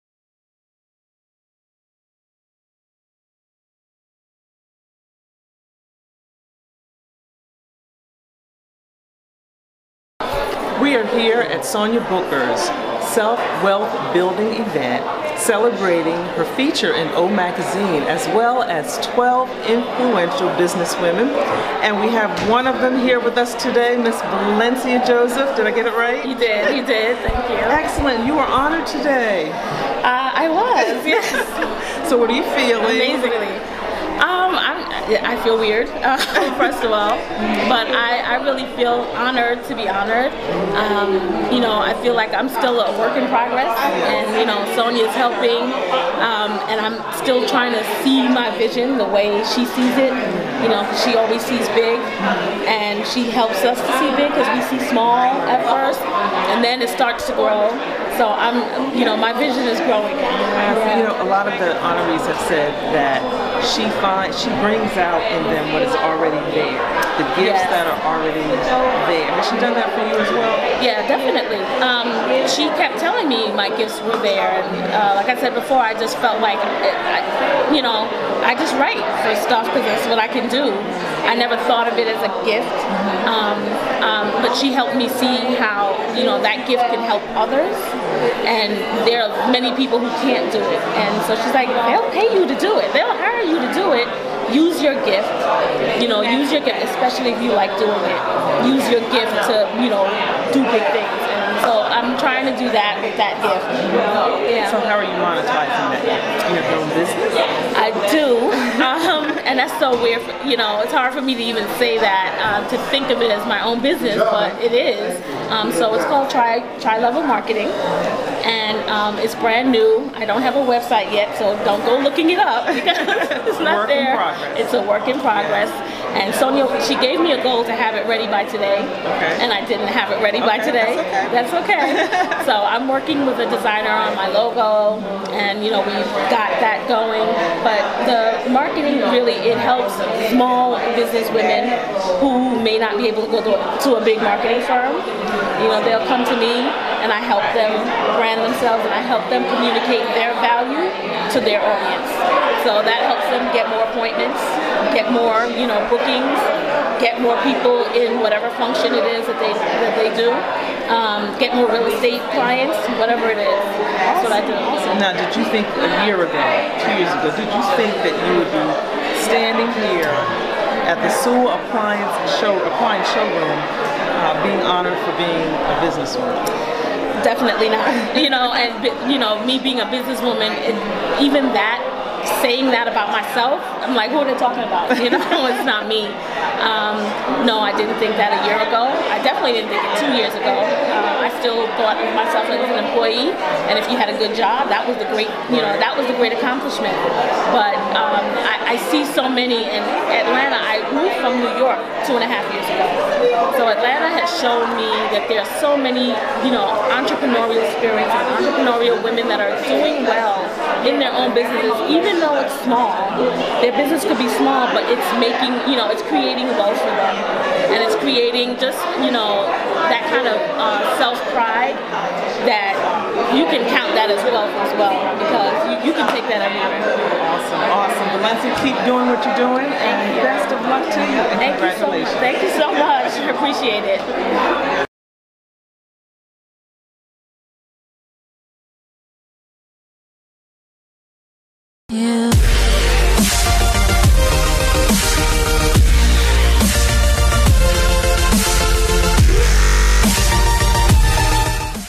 Women Making History Valencia Joseph
Sonia Booker's Self Wealth Building event celebrating her feature in "O" Magazine as well as celebrating 12 up and coming influential business women.  Inspiring!